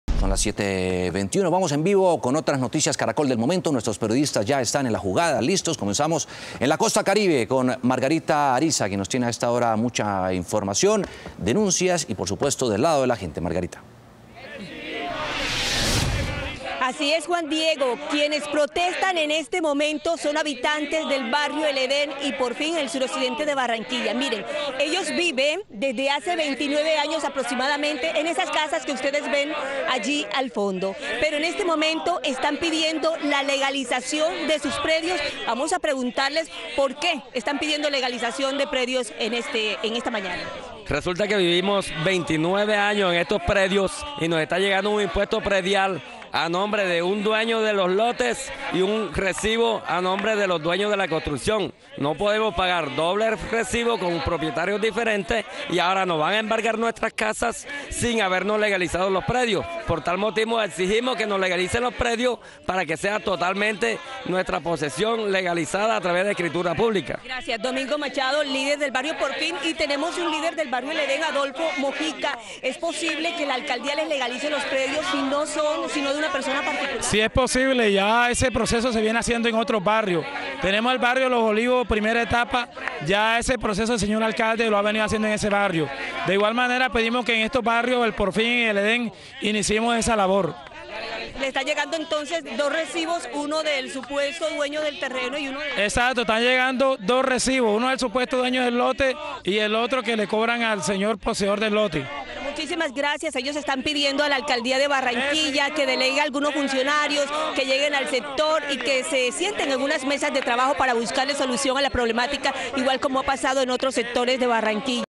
Les cobran dos impuestos prediales y las propiedades en Barranquilla ni siquiera están a sus nombres
Habitantes de los barrios El Edén y Por Fin piden legalizar el estado de sus viviendas.